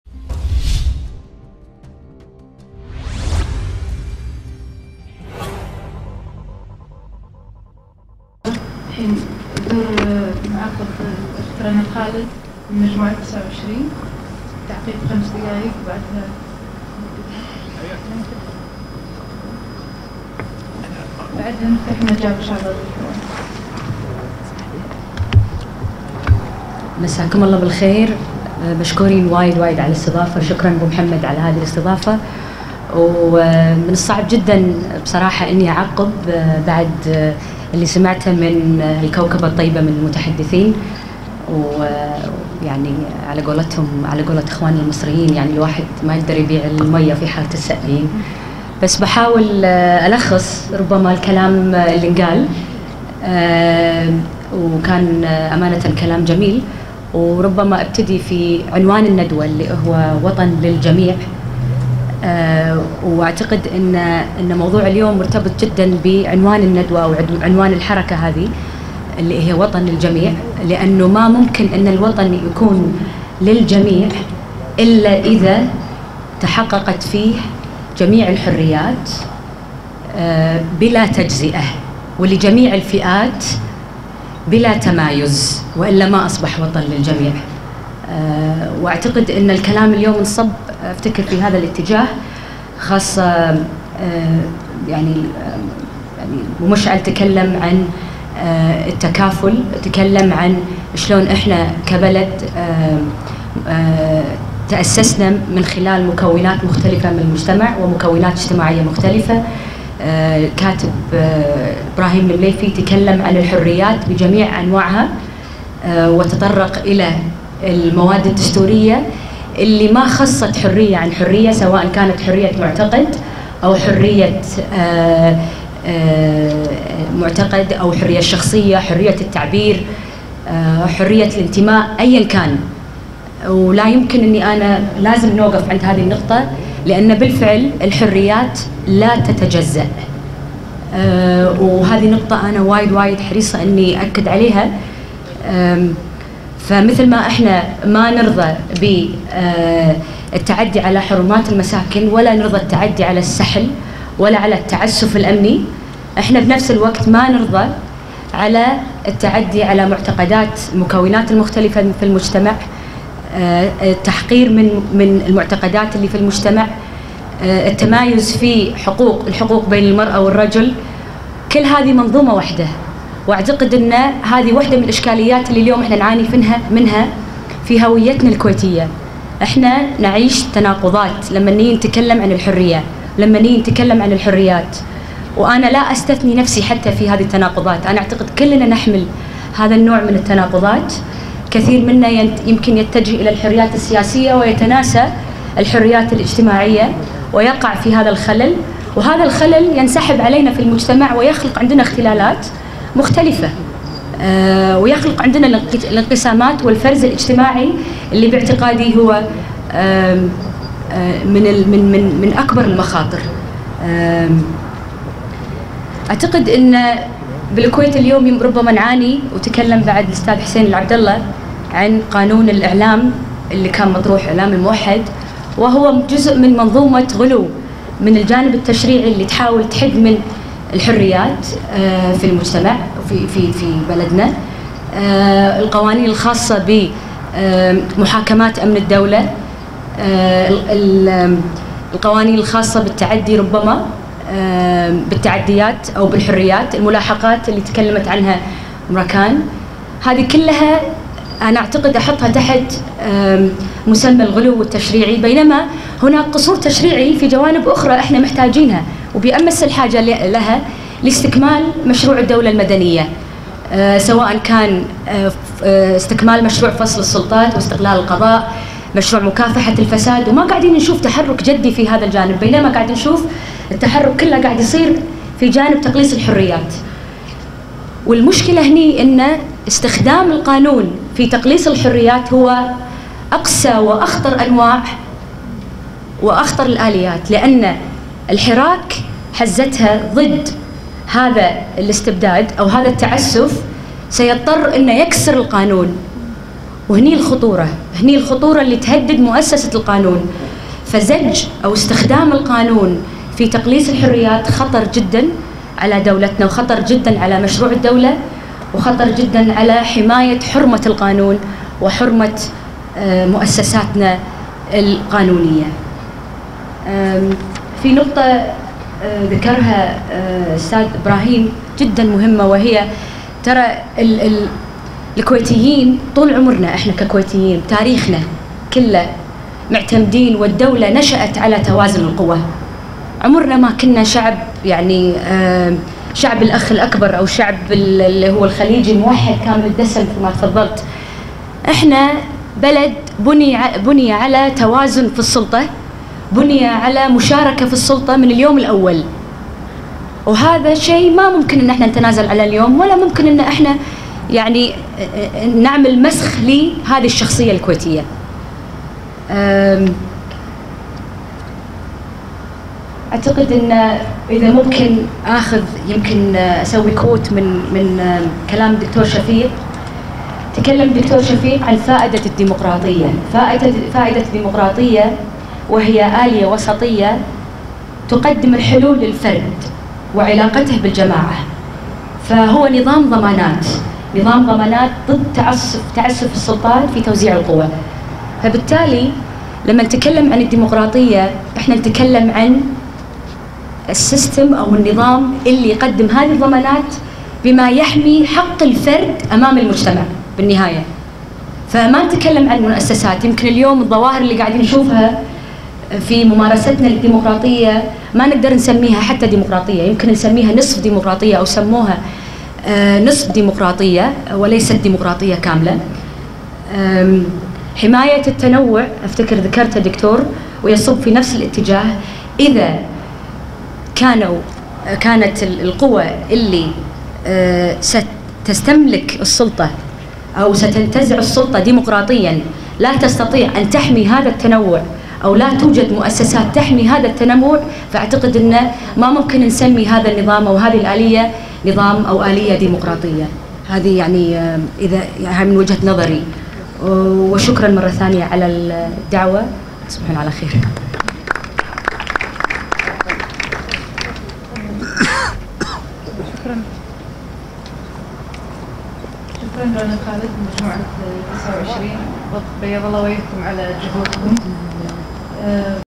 رنا الخالد من الجلسة الحوارية (وطن للجميع) في ديوان صالح الملا | الجلسة الـ1 العدالة والحريات
فيديو: رنا الخالد عضو "مجموعة 29" من الجلسة الحوارية (وطن للجميع) التي نظمها صالح محمد الملا في ديوانه بمنطقة شرق | الجلسة الأولى العدالة و الحريات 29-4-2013 ..